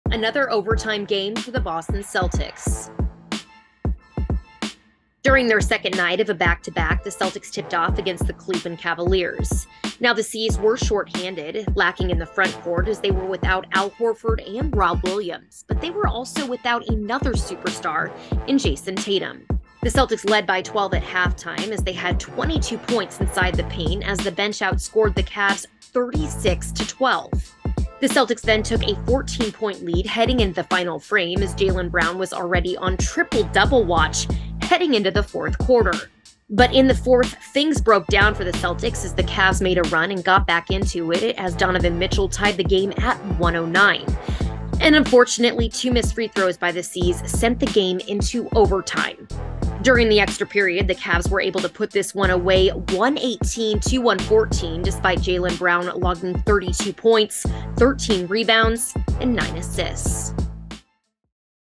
3/6 Putnam Postgame Report: JB Fills the Void
Jaylen Brown stepped up for the shorthanded Celtics on Monday night, but fatigue may have gotten the best of the C’s on the second night of a back-to-back.